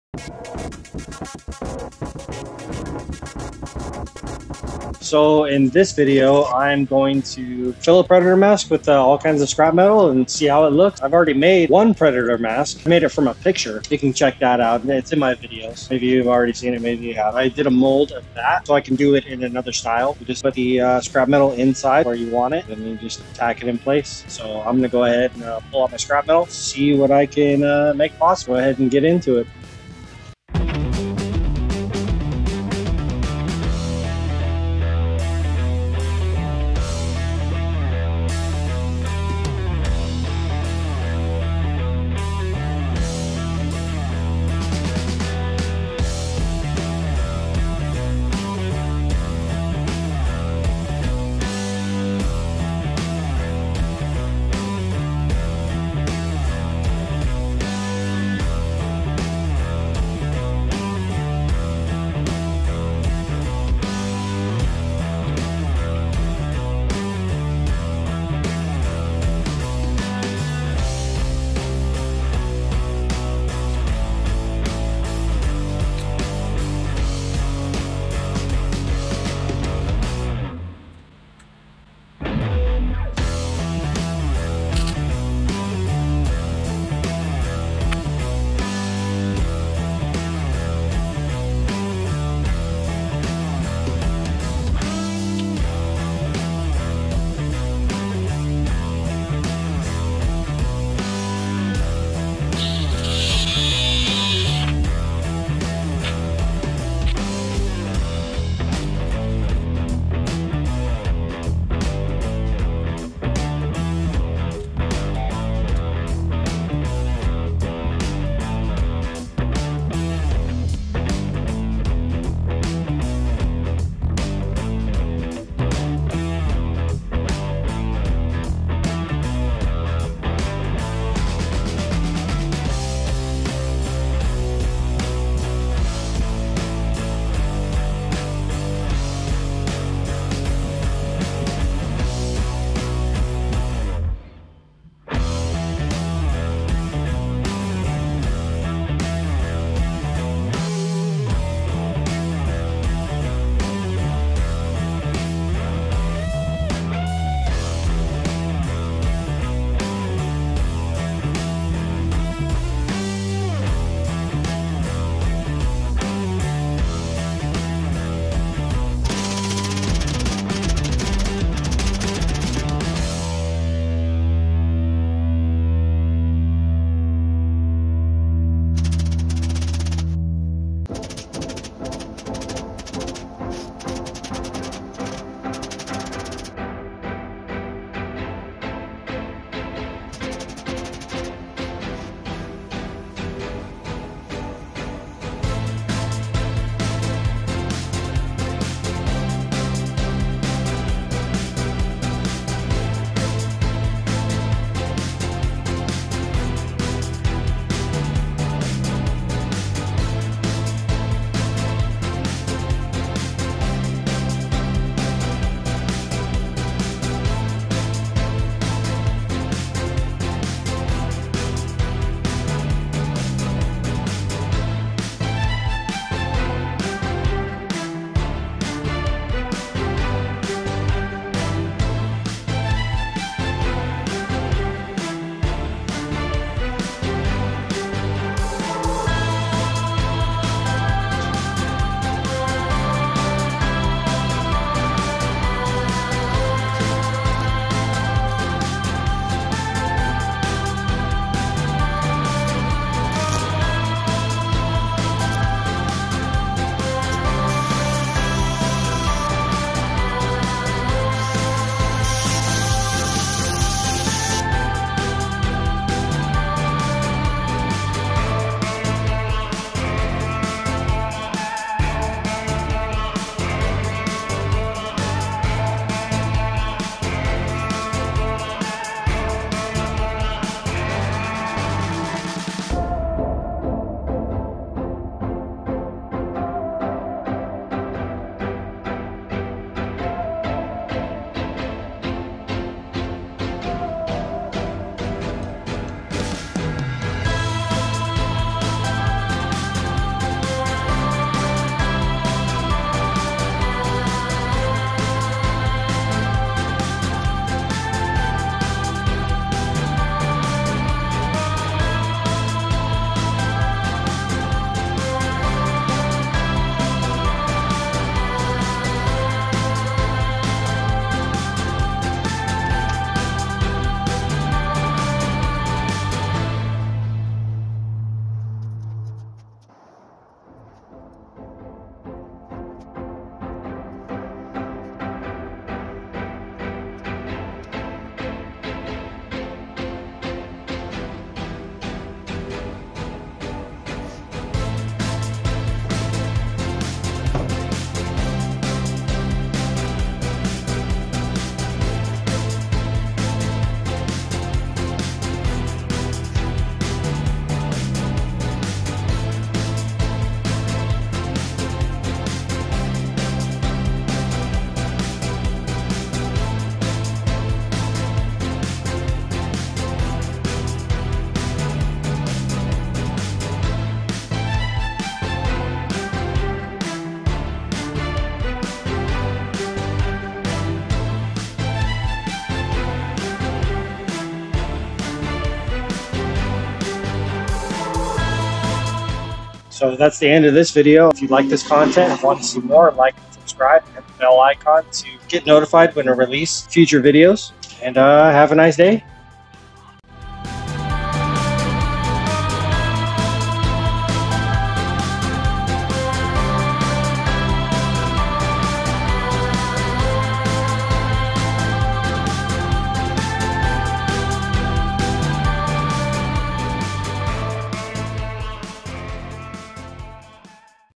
Scrap metal Predator mask using a plaster mold.
I made a plaster mold from the first predator mask I originally made from a drawing and organized metal inside of the mold to weld it all together. using parts from different types of machinery.
 some of the parts used but not limited to:

-spark plugs and wires
-hole punch slugs
-engine valves
-transmission ring gear
-7.62x39 bullet cases
-223 bullet cases
-aircraft seat adjustment motor gears
-chainsaw chain

To weld I use a miller 211 wire feed with solid wire and 75/25 shielding gas. powered by a cummins inverter generator